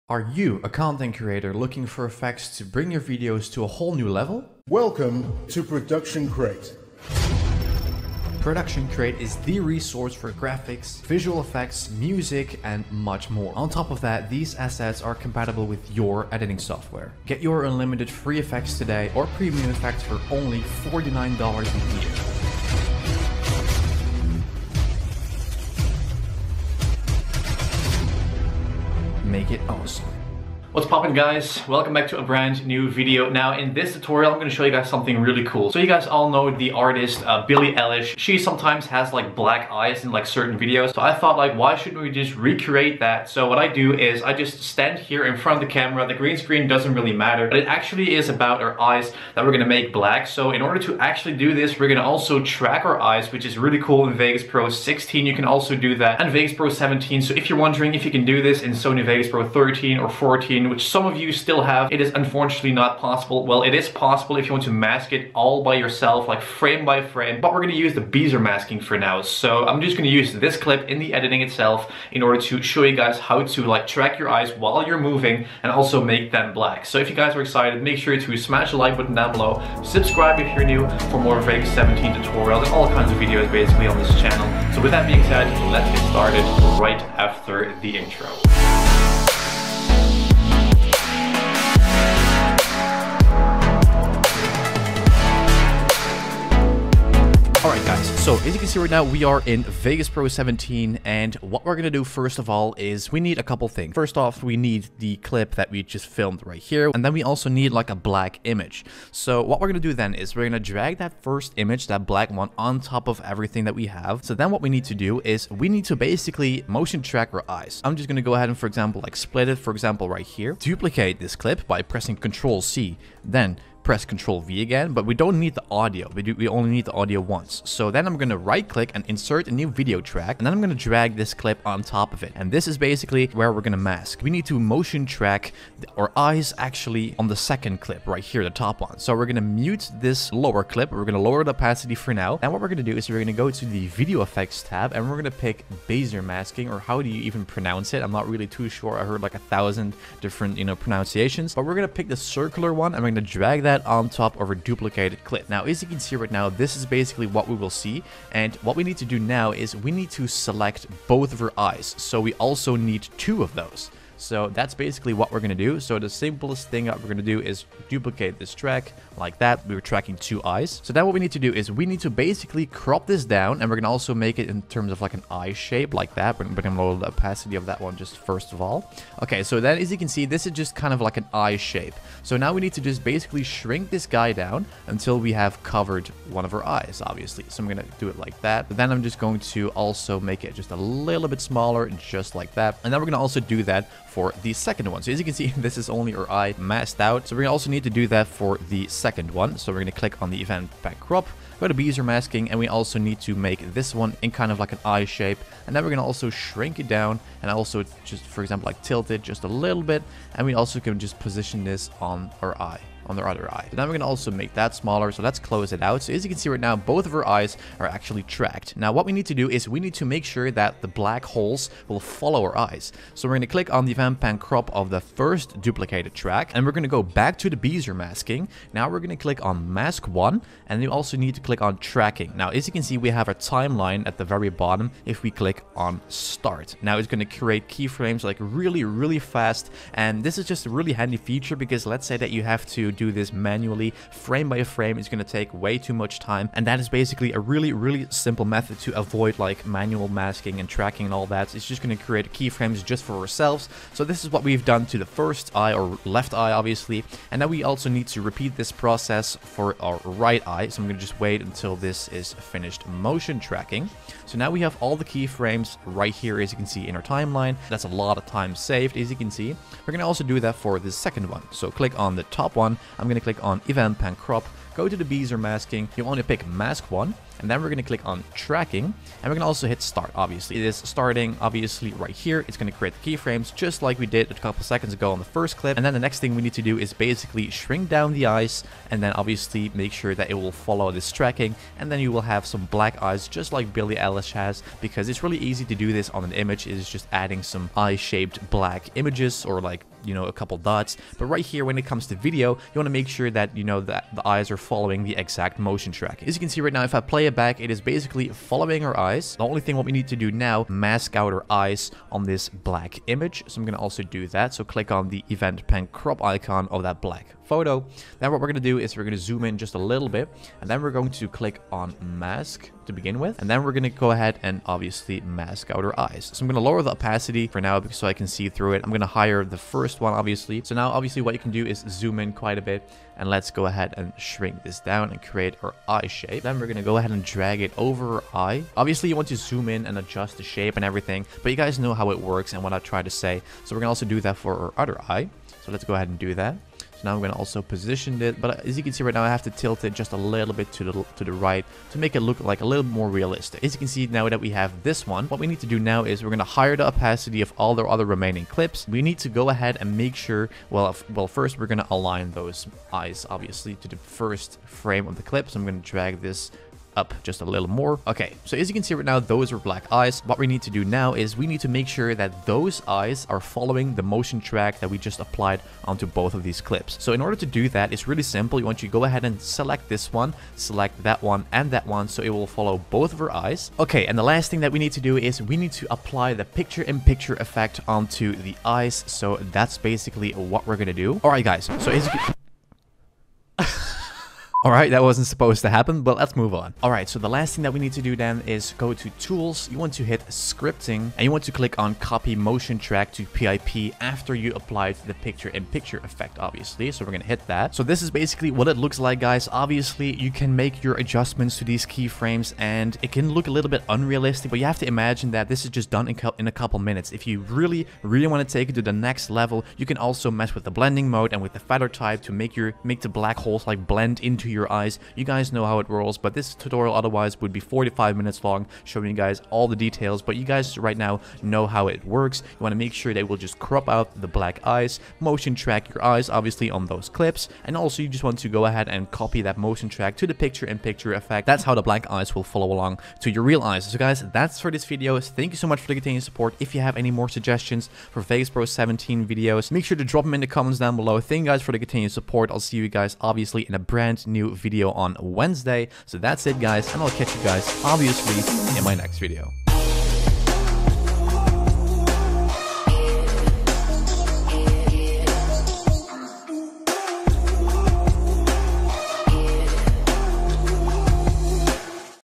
VEGAS Pro 17: How To Create Black Eyes Like Billie Eilish - Tutorial #457
- - Today I will be showing you how to create black eyes like Billie Eilish in VEGAS Pro 17! Make sure to SMASH that like button below as well! Stick around for more content!